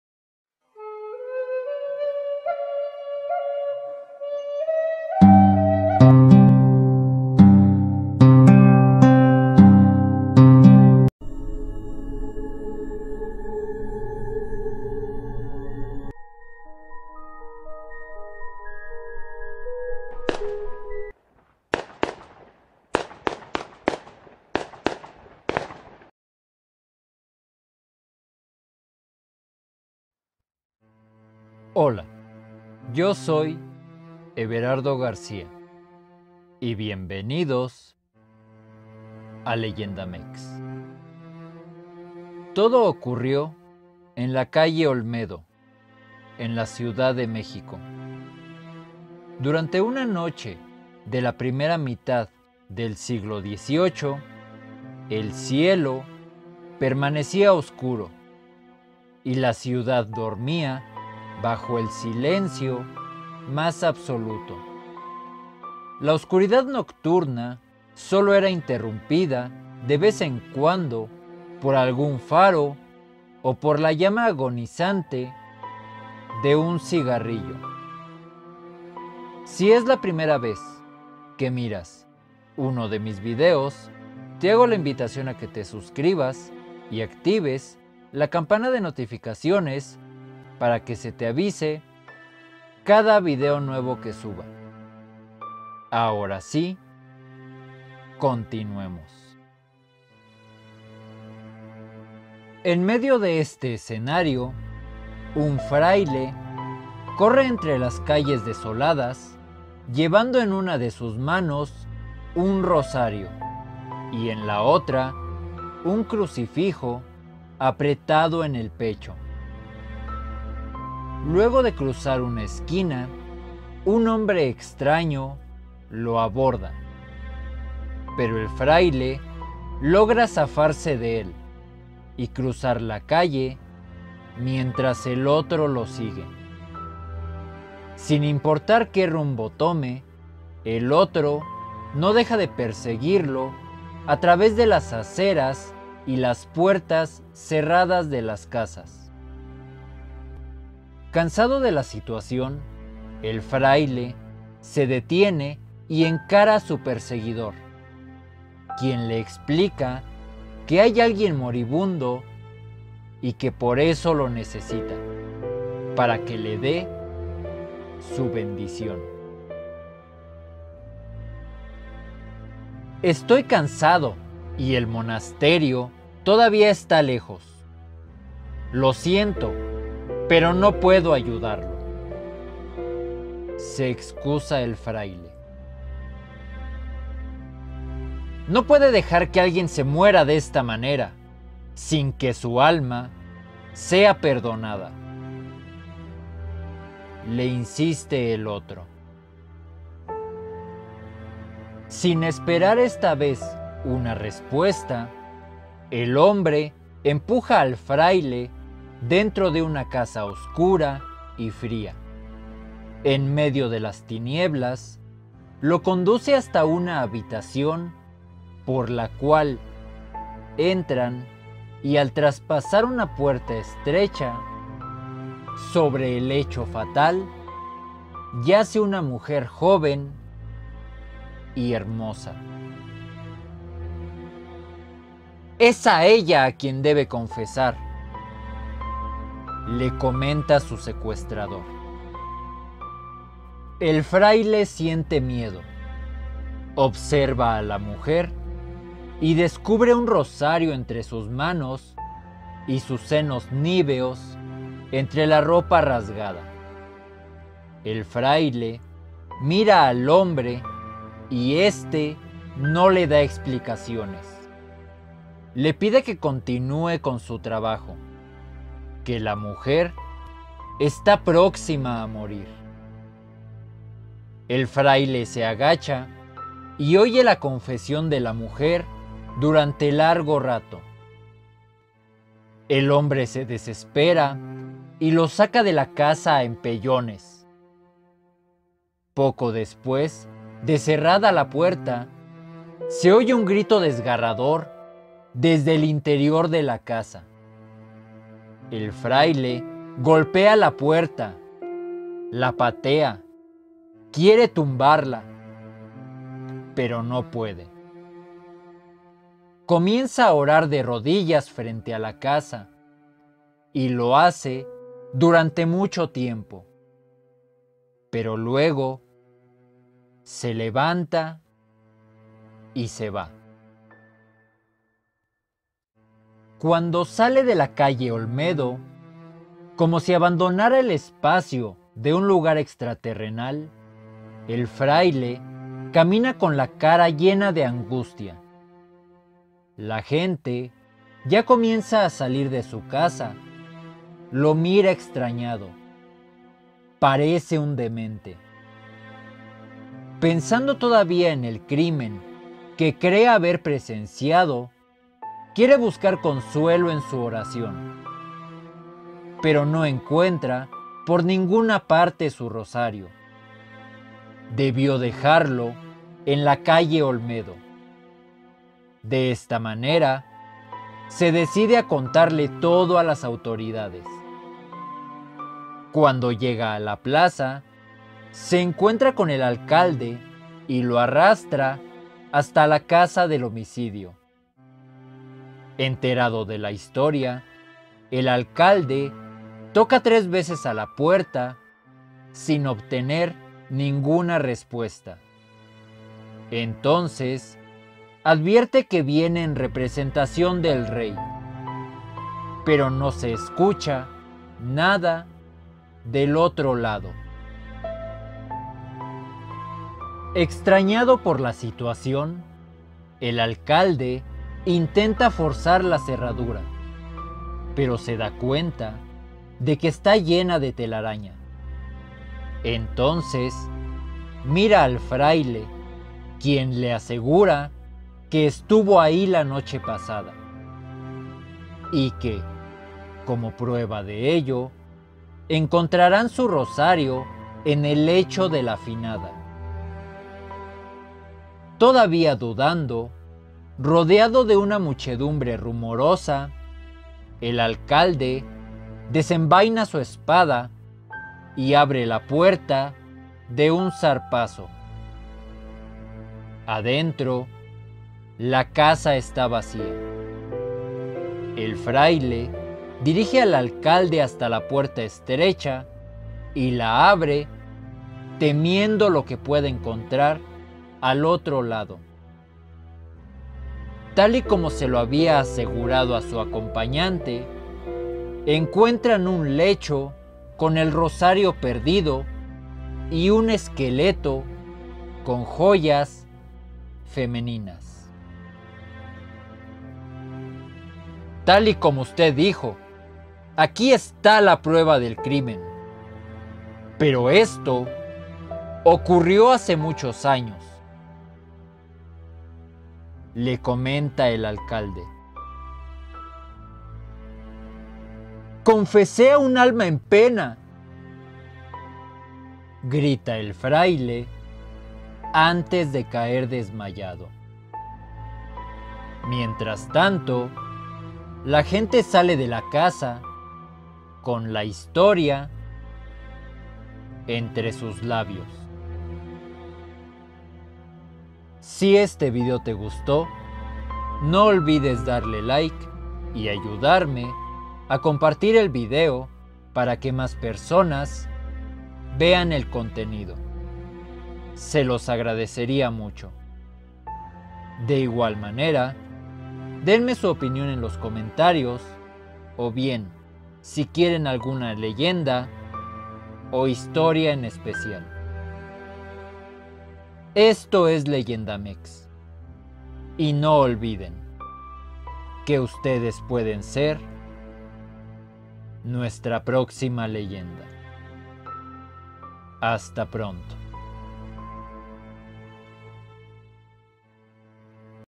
La calle Olmedo | Leyendas tradicionales y coloniales
Hola yo soy Everardo García y bienvenidos a Leyendamex...
En esta ocasión les contaremos una leyenda donde una vez mas queda demostrado que si un alma necesita ayuda de nosotros buscará la manera hasta que alguien le ayude.